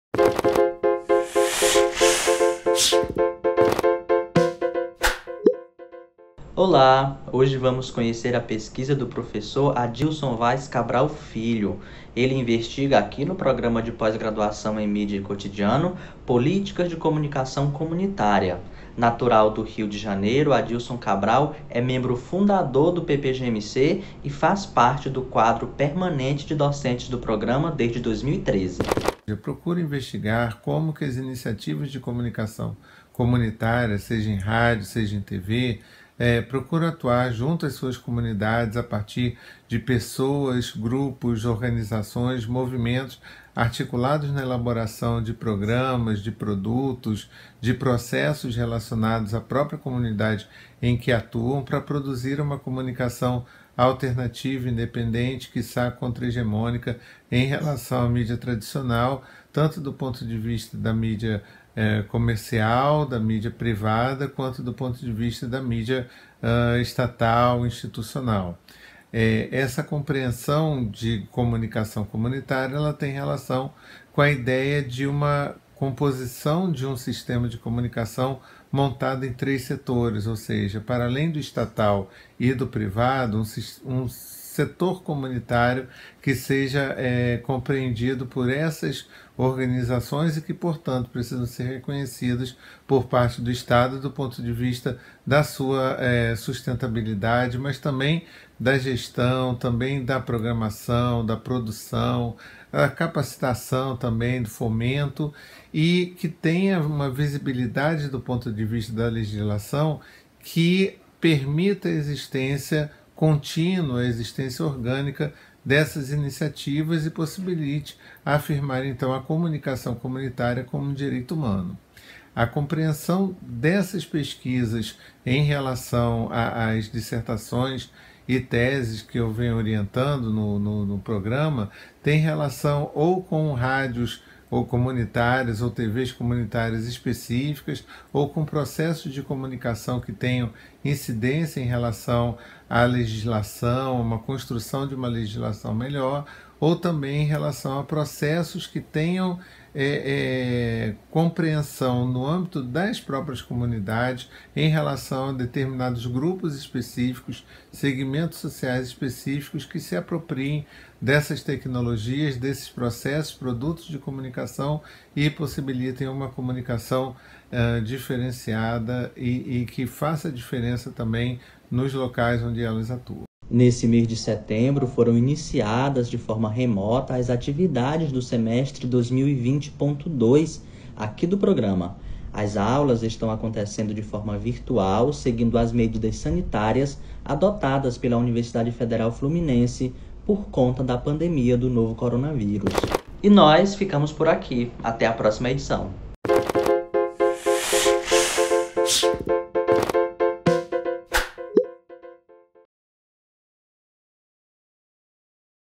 MinutoCotidiano | Prof. Adilson Cabral
🧡 Apresentamos um episódio histórico do Minuto Cotidiano!

🗣 O convidado desta edição é professor Adilson Cabral, um dos fundadores do PPGMC UFF. Ele fala sobre a pesquisa que desenvolve no Programa envolvendo o tema "Políticas de comunicação comunitária".